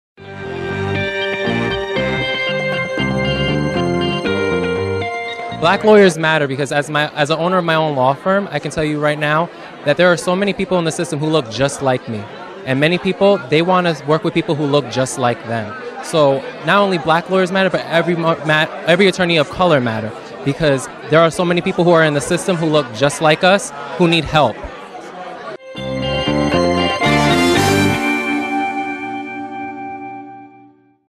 MBBA BlackLawyersMatter Series: John Raphael Pichardo
John-Raphael Pichardo shares his thoughts on why Black Lawyers Matter.